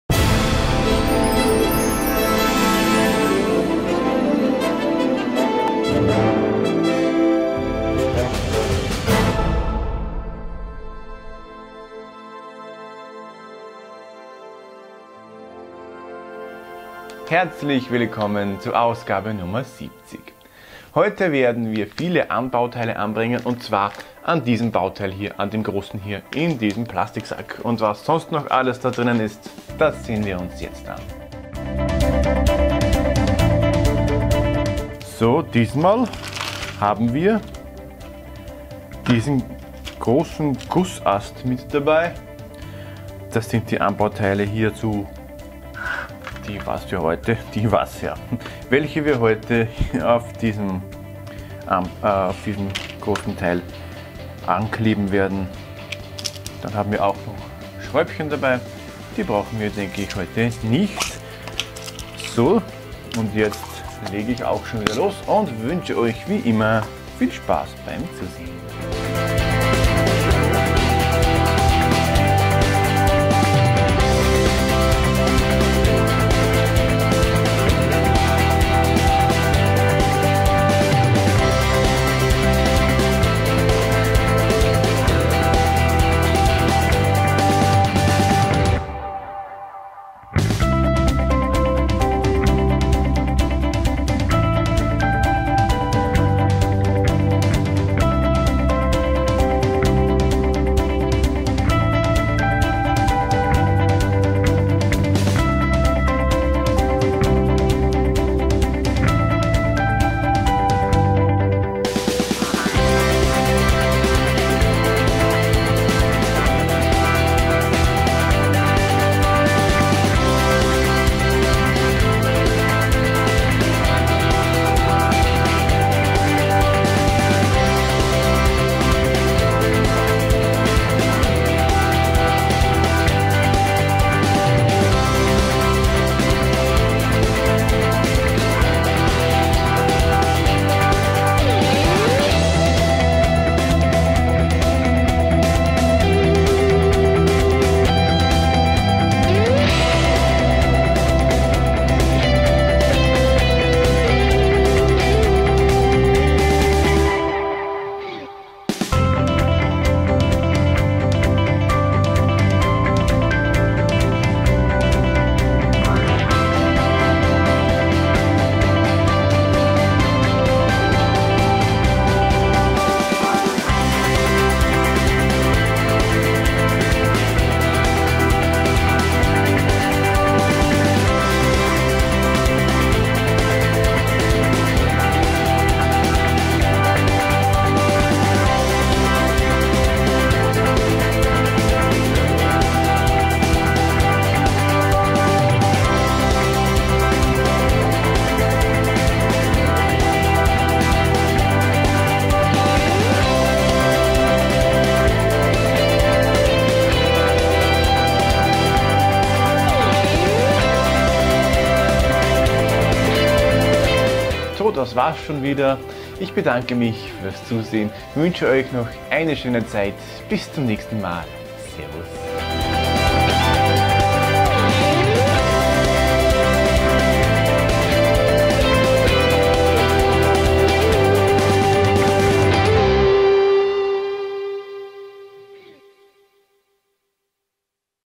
Build The Millennium Falcon - DeAgostini - #70
Ich baue mit euch den Millennium Falcon aus Star Wars - Episode V von DeAgostini. Jede Woche gibt es eine neue Folge mit Bauberichten und auch Specials. Über ein Abo würde ich mich sehr freuen.

// Licences //